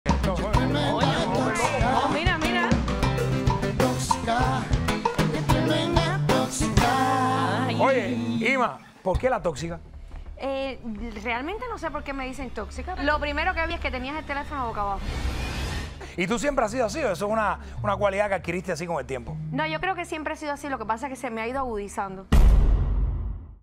Imaray Ulloa confiesa lo de tóxica con Mijail Mulkay este martes a las 8pm
UniVista TV